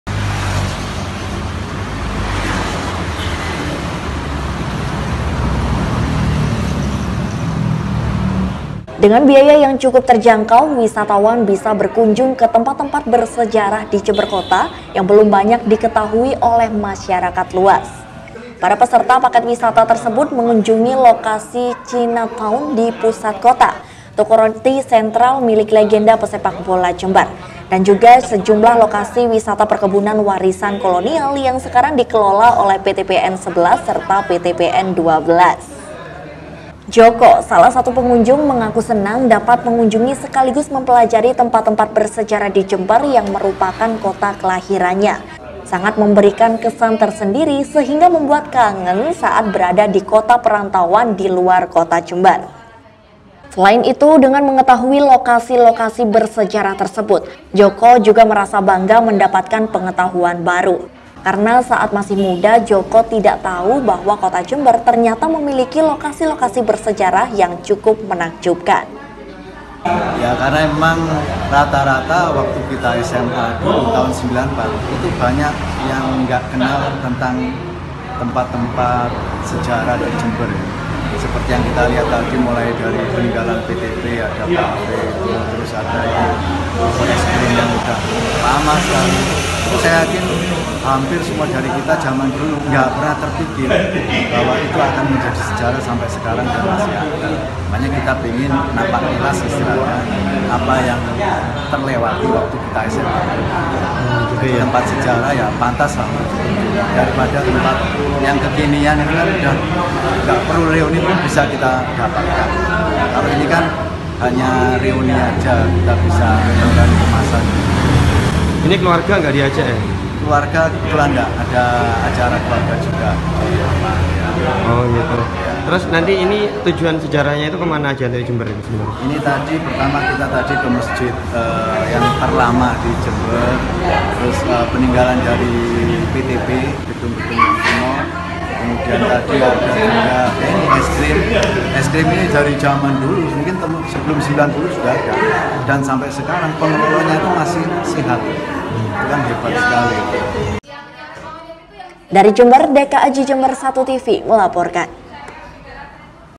Wisata Sejarah Kota Jember
Di Jember terdapat paket wisata alternatif sangat murah dengan mengunjungi lokasi-lokasi bersejarah, cukup dengan biaya 250 ribu rupiah per-angkutan umum atau angkot/ pemudik dapat menikmati kembali kenangan saat-saat masih tinggal di kota tembakau ini.